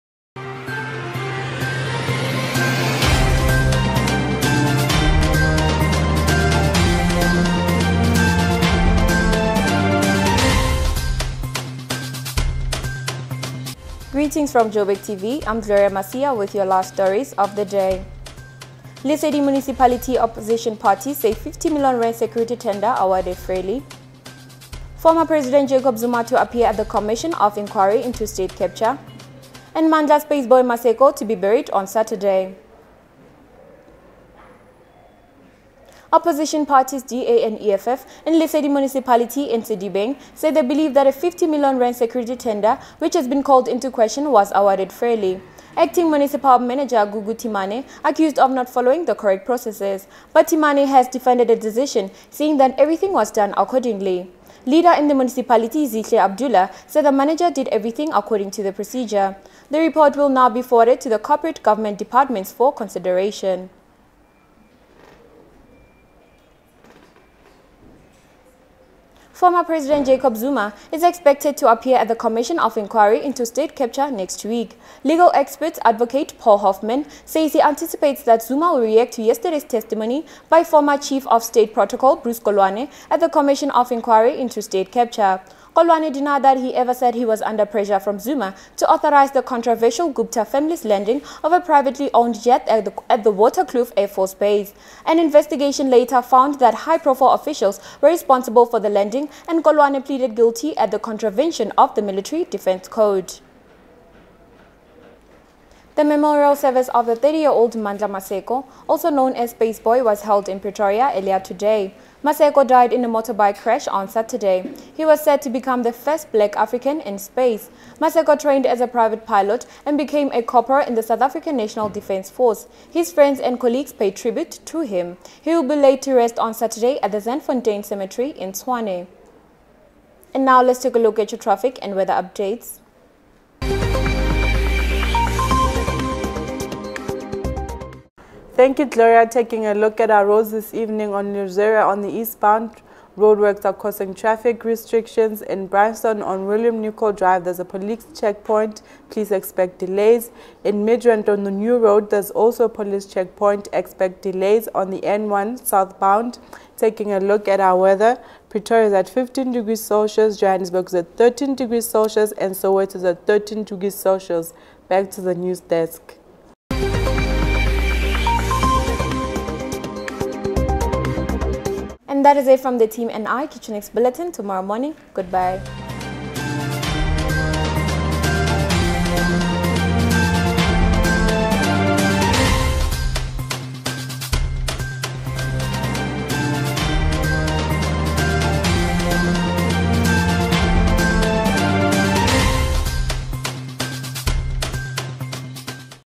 Evening Bulletin - Joburgtv News - 10 July 2019
South Africa and the  Rugby world laments the death of  Springbok winger James Small.
Small, who was a member of the 1995 Rugby World Cup winning squad, died suddenly from a heart attack in a Johannesburg hospital.

The 50-year-old Small was rushed to hospital in Johannesburg on Tuesday night after suffering from a suspected heart attack.